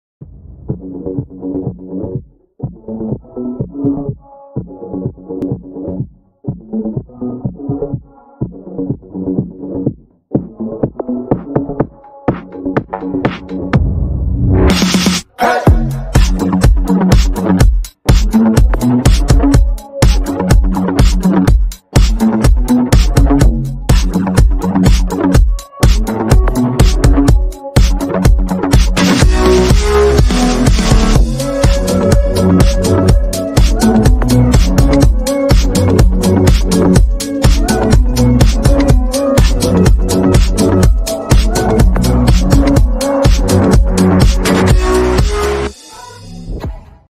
Pump track at Nokhook skate park
Lily celebrate her 10th Years old Birthday at Nokhook Skate park